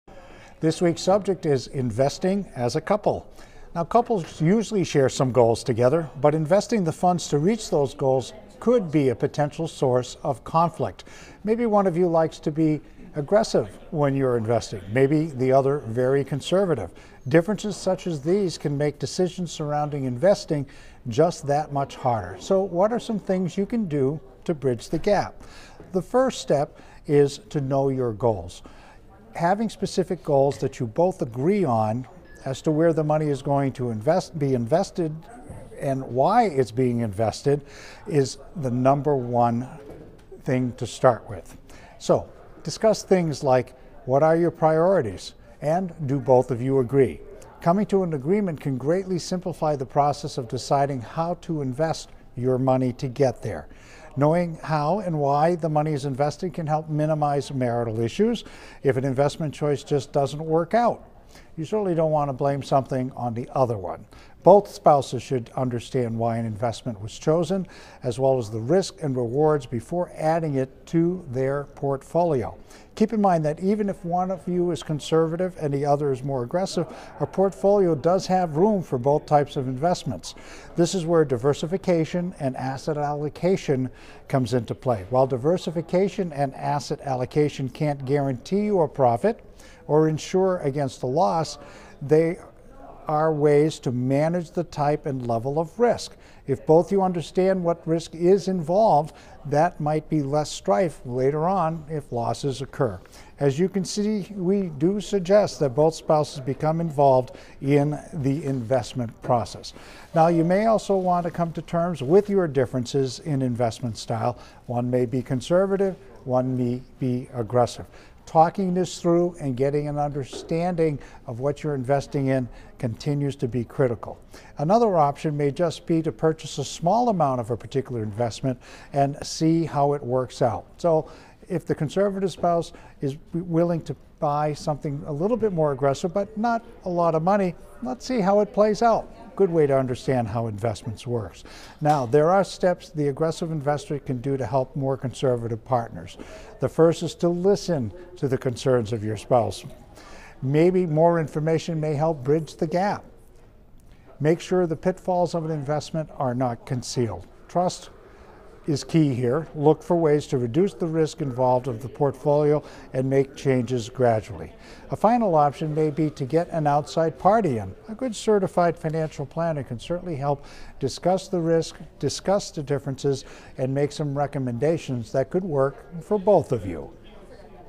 Money Matters: Investing as a couple
Couples usually share some goals, but investing the funds to reach those goals might be a source of conflict.